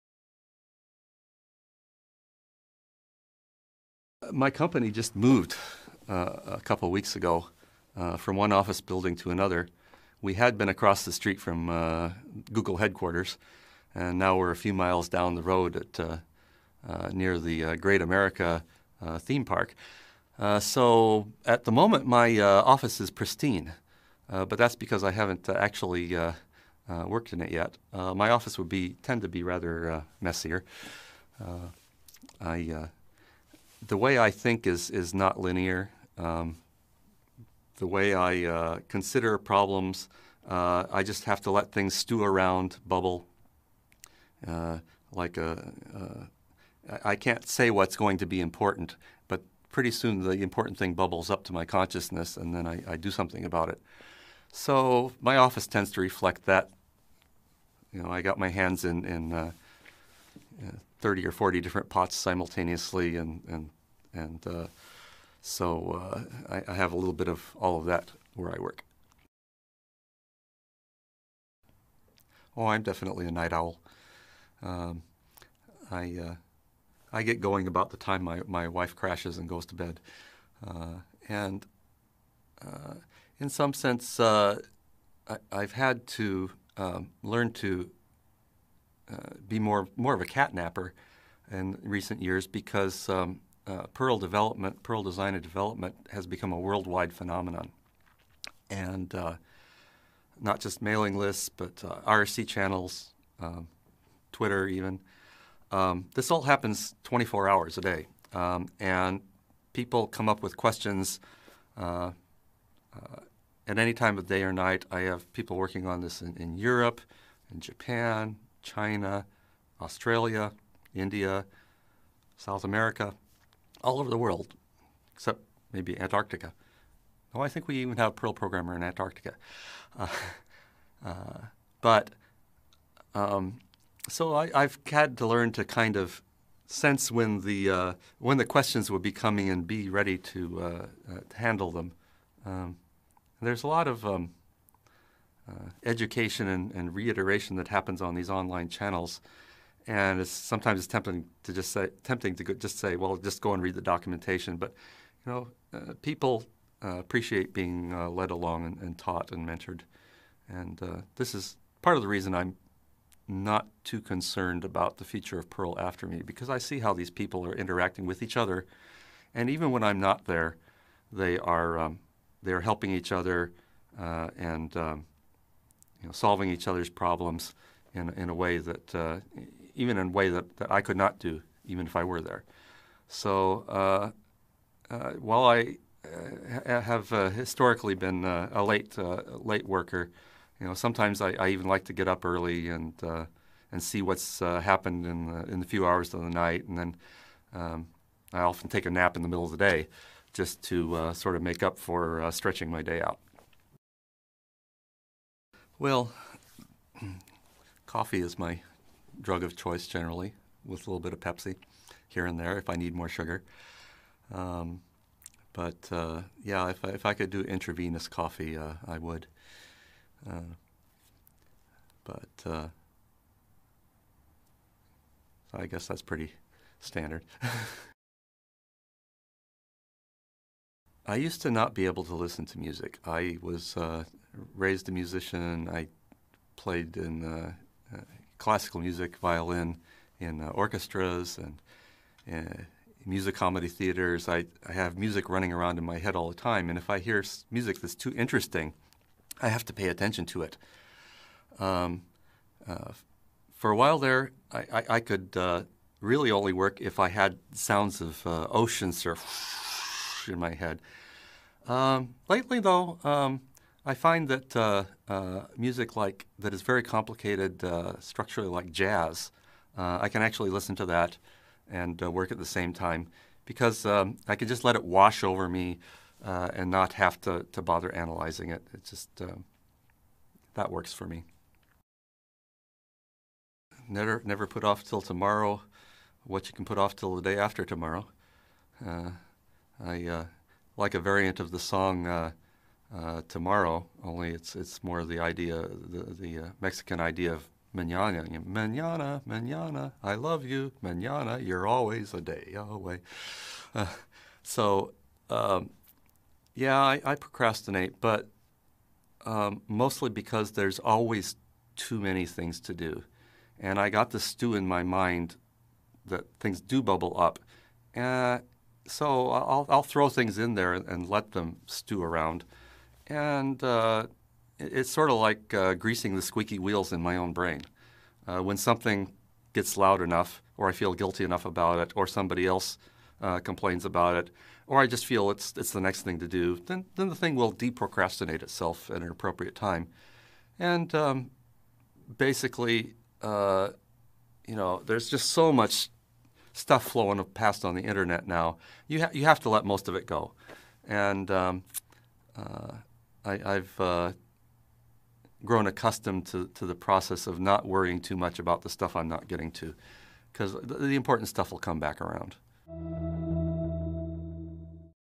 How to Code Like Larry Wall (creator Perl Programinh language). Subtitle Indonesia.
Larry Wall adalah seorang ahli di bidang ilmu komputer dan linguistik, dan mempunyai minat besar terhadap seni dan filsafat. Gabungan keahlian dan minat inilah yang memberi pengaruh besar pada rancangan Perl, dan juga budaya yang berkembang di sekitarnya. Wikipedia